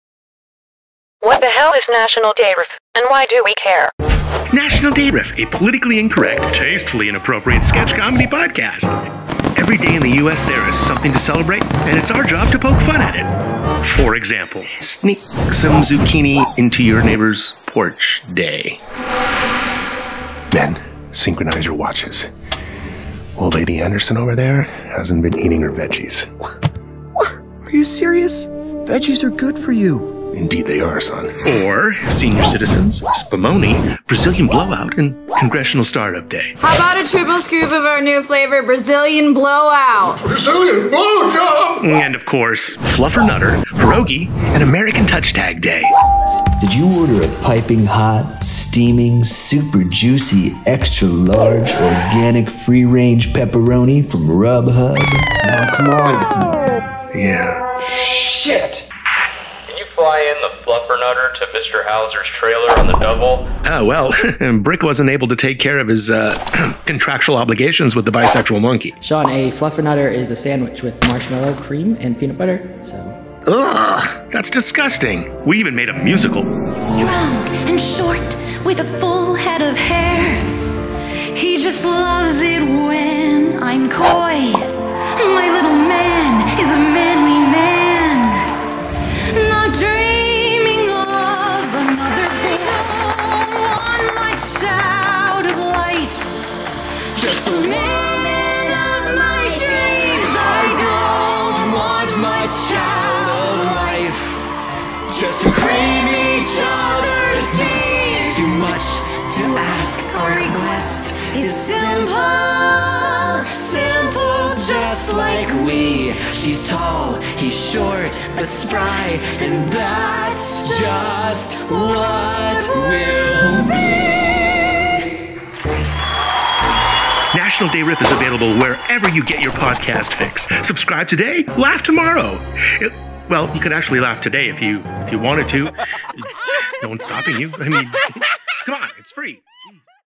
What the Hell is National Day Riff and Why Do We Care?
National Day Riff is a politically-incorrect, tastefully-inappropriate sketch comedy podcast, available wherever you get your podcast fix.

Hosted by Sean Michael Beyer and starring the voices of Nick Pasqual, Adam Carbone, and Angela Hicks.

Written & Produced by Sean Michael Beyer. Audio Engineer, Sound Mix & Design by Shadow Mercury.